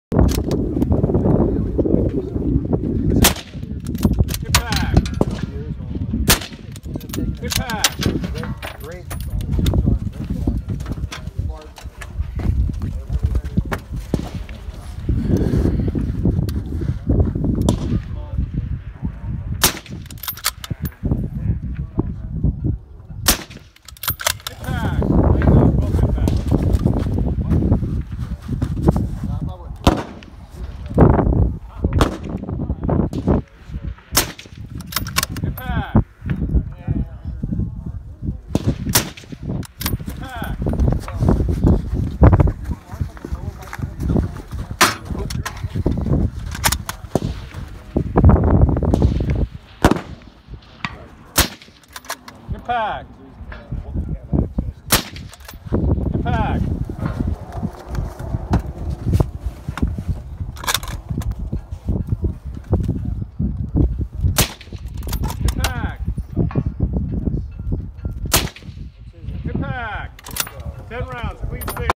Camp Pendleton Precision Rifle Shoot
Camp Pendleton Marines and members of the Santa Margarita Gun Club participated in a precision rifle shoot March 3rd and 4th. The Shoot was hosted by the School of Infantry.